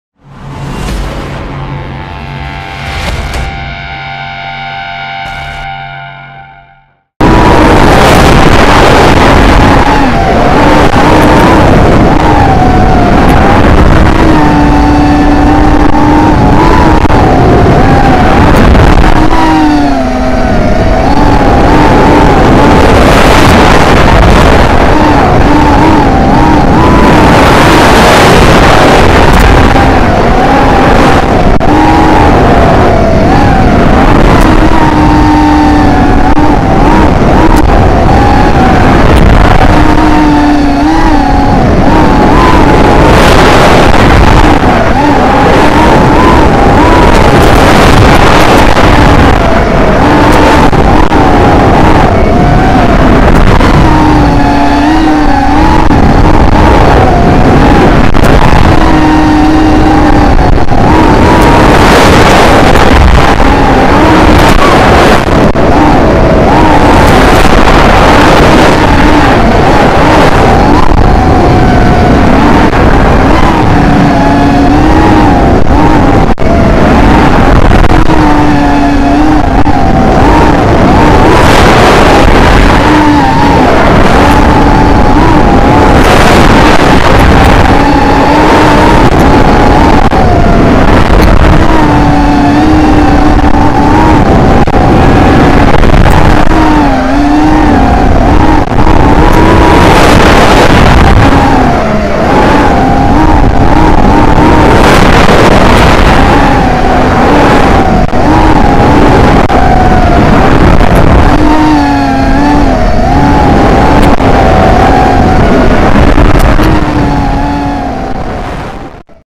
RACEFLIGHT // BB204 (TEST1) // UH32
Raceflight // BB204 // TEST1 ON
Frame: B.One V5
FC: KKNGF4
IMU: MPU6500
UBAD 30A // Multishot 14.6
Emax Red Base 2300KV
Control loop: UH32
Gyro: 32 Khz
Motors write: 32 Khz
Pwm update: 32 Khz
Props: DAL 4x4x3